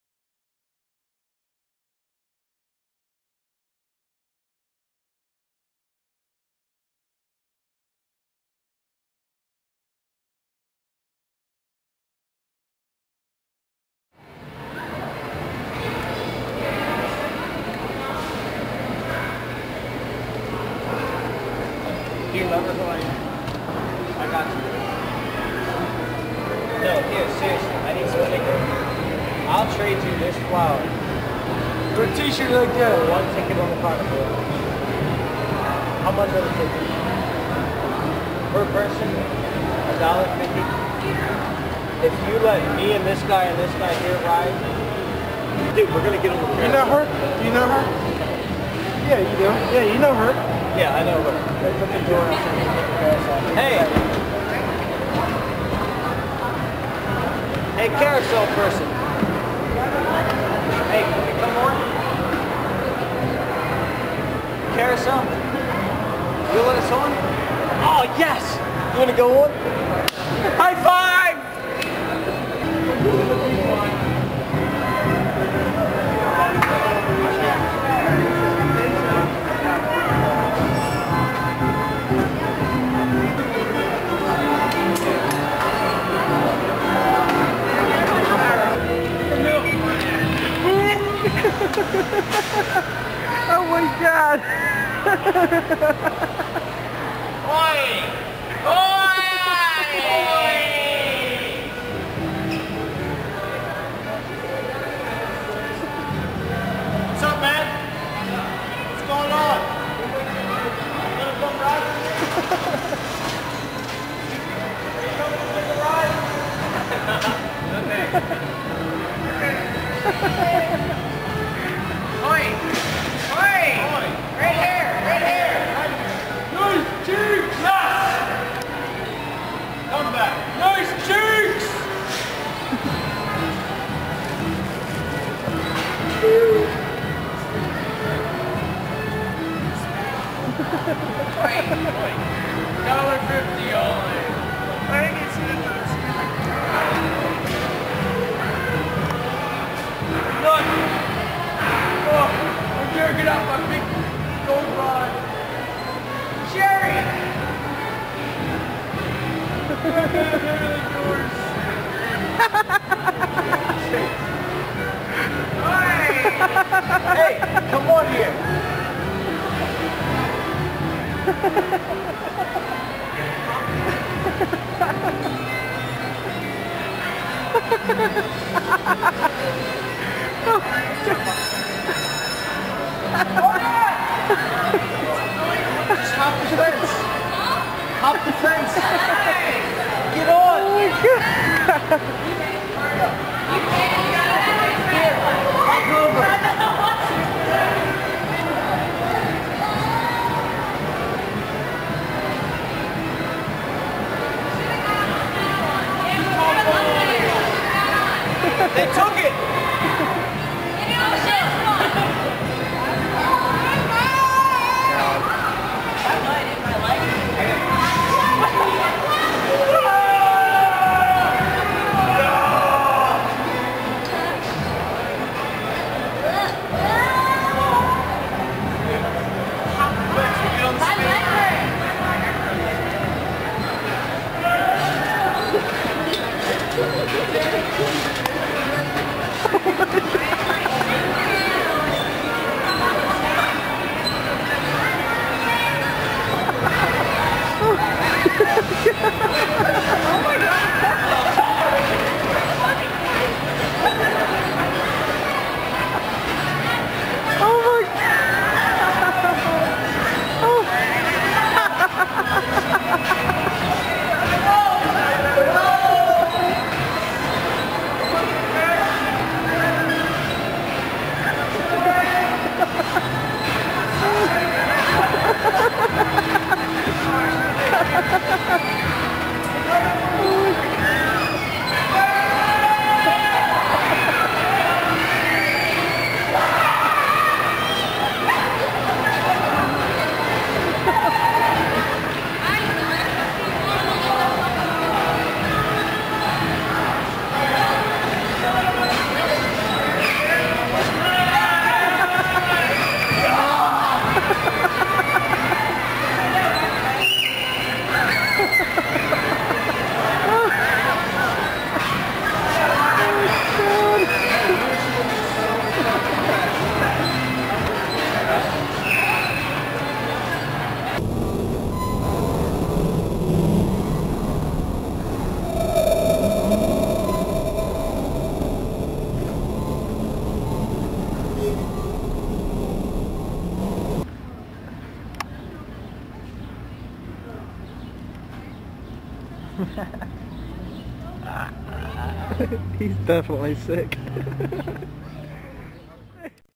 Carousel
Part of The Missing Tape Series, Dub and Phoenix take a ride on the carousel at the Mall in Columbia and Dub eventually gets sick